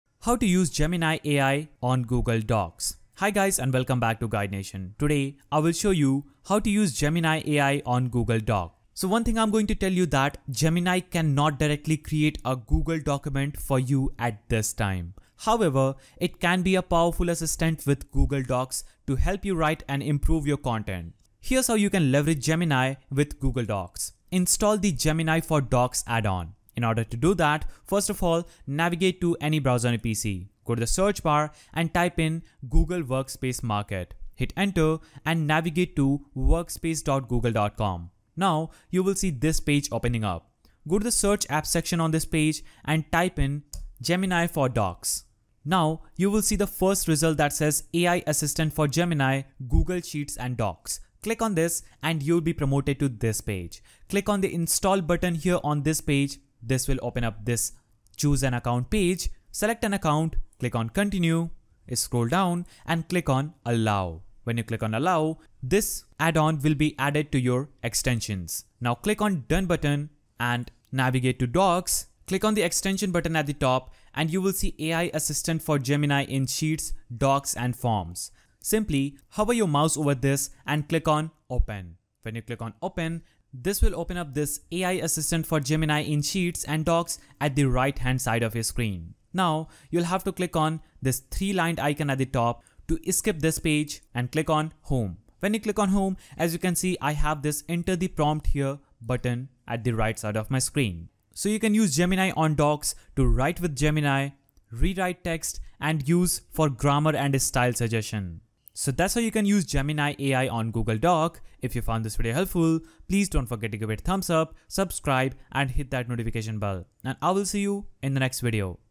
How to Use Gemini AI on Google Doc (SIMPLE & Easy Guide!)
Learn how to use Gemini AI on Google Docs with this simple and easy guide! In this video, we'll walk you through the steps to integrate Gemini AI into Google Docs, enhancing your writing and editing process with advanced AI tools. Perfect for beginners and experienced users looking to boost their productivity and create high-quality documents. Follow these tips to effortlessly use Gemini AI on Google Docs!